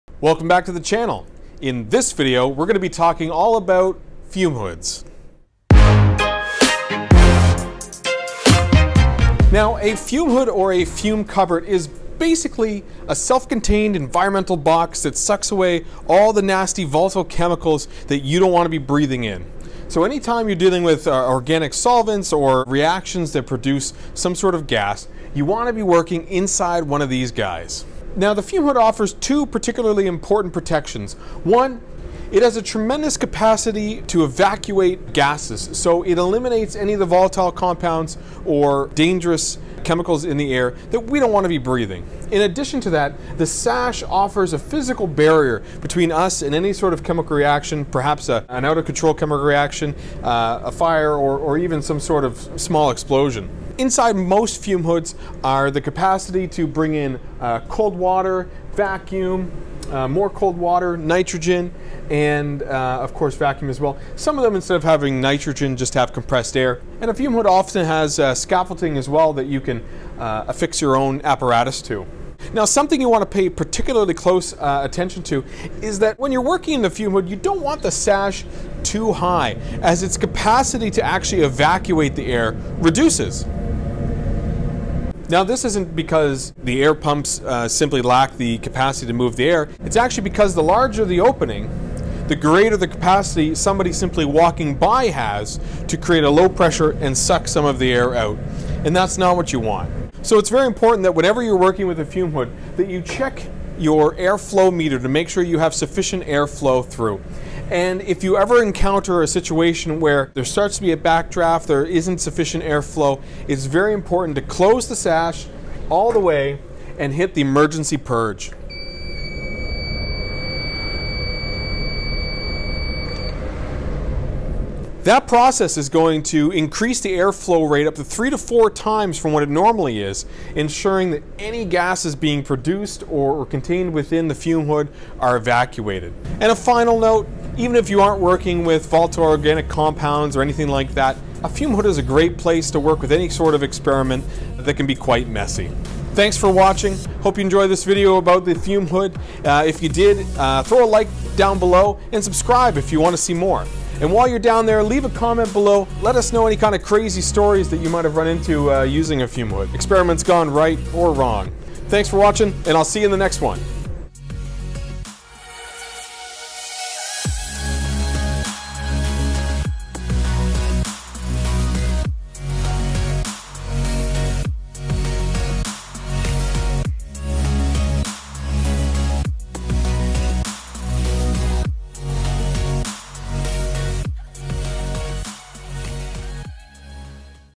How To Use a Fume Hood
In this video, I explain how to properly use a fume hood.